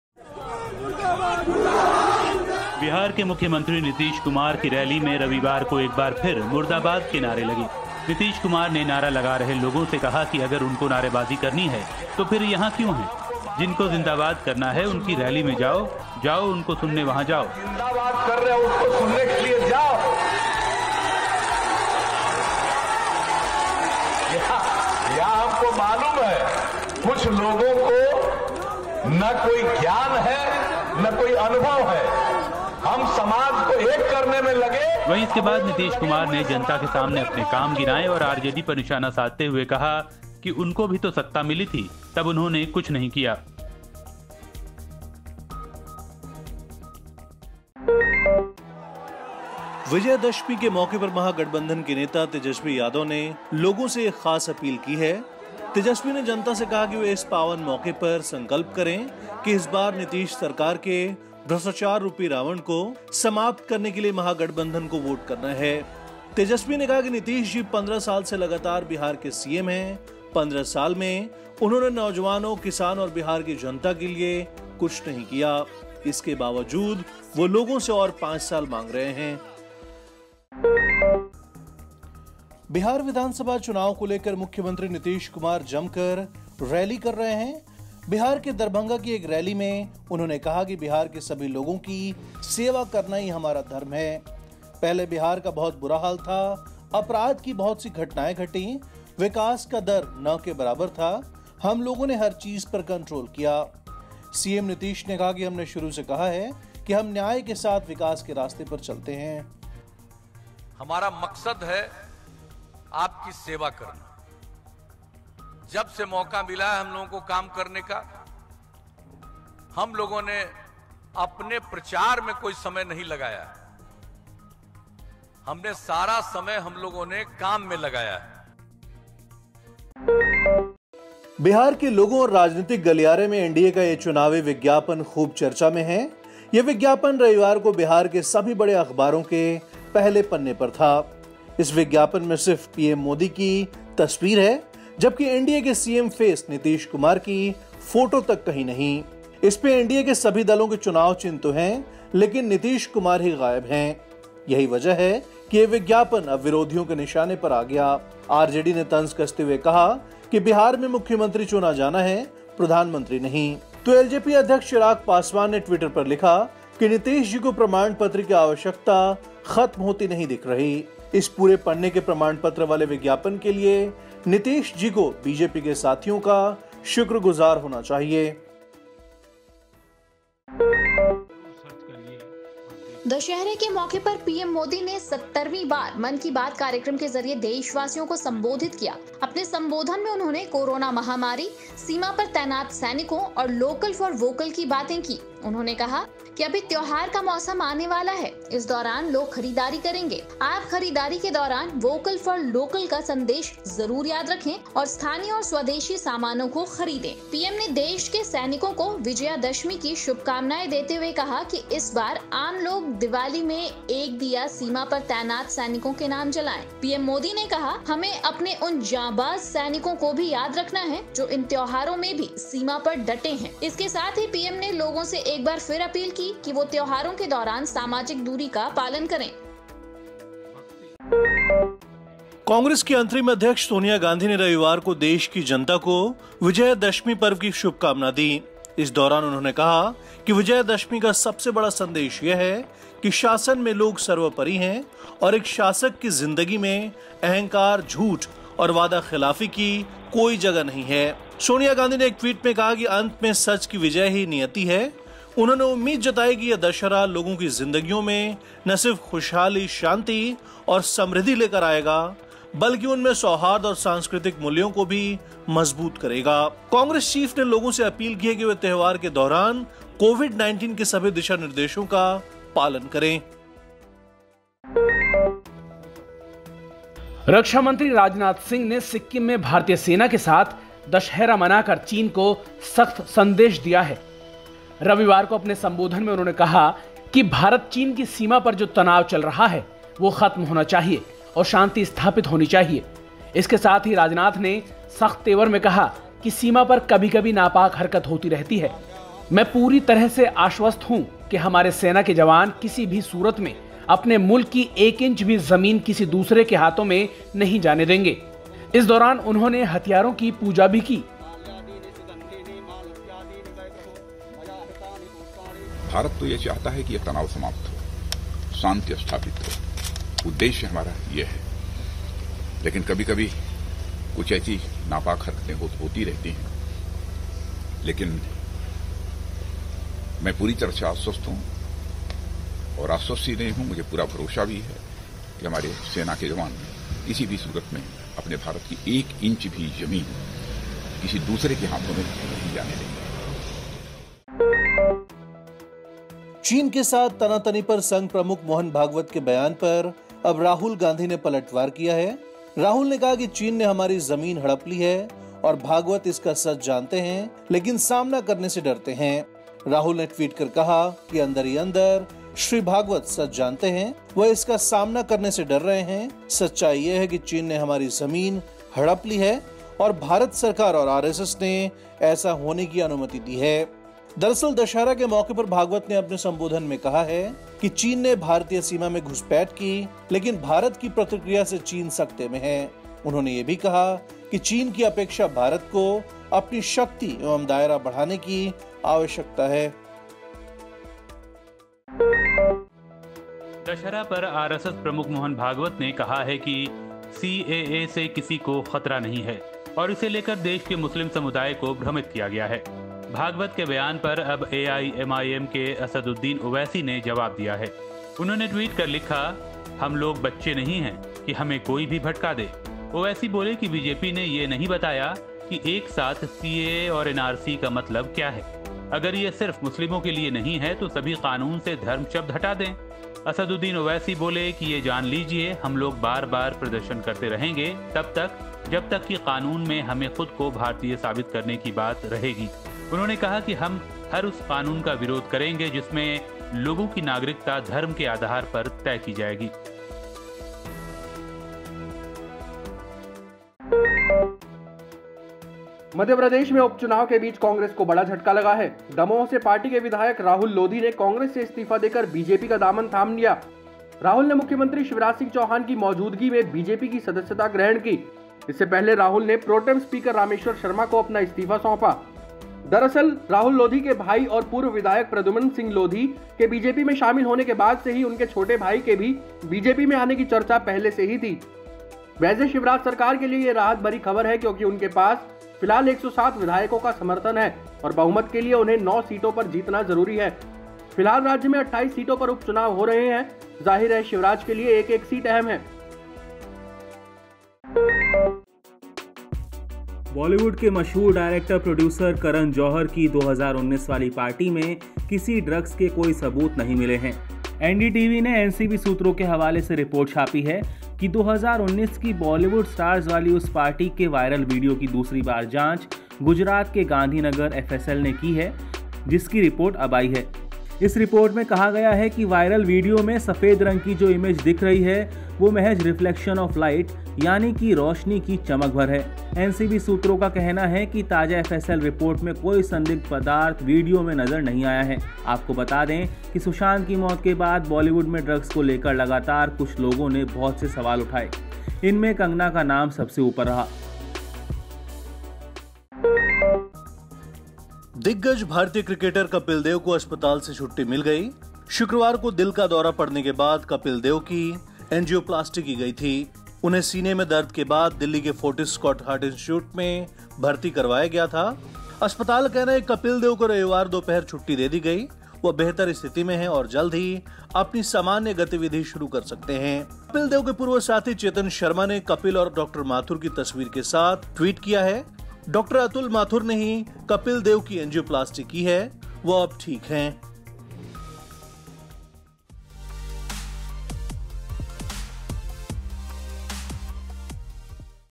नीतीश की रैली में फिर हुई नारेबाजी तो दशहरा पर तेज हुई सियासत, बड़ी खबरें देखिए एडिटरजी पर
28 तारीख को पहले चरण के मतदान से पहले बिहार में चुनावी सरगर्मी चरम पर है. रैलियों में आरोप प्रत्यारोप के साथ साथ वादों की झड़ी लगी है. मौका दशहरा का है तो सियासी बाण भी खूब चल रहे हैं. देखिए रविवार शाम की बड़ी खबरें एडिटरजी की प्लेलिस्ट में